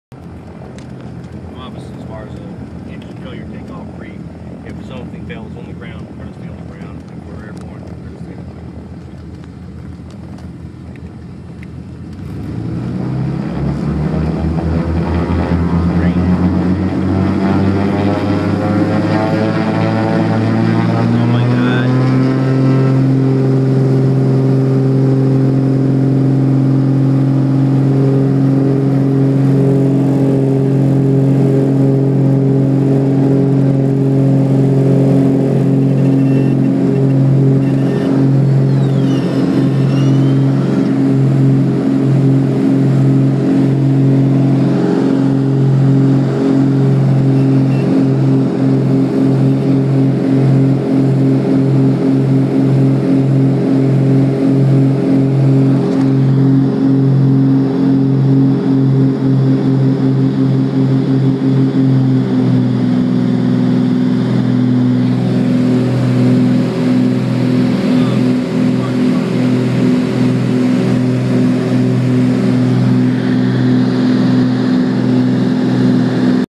Sitting in the back seat of a E58 baron during takeoff
Taking off from CLW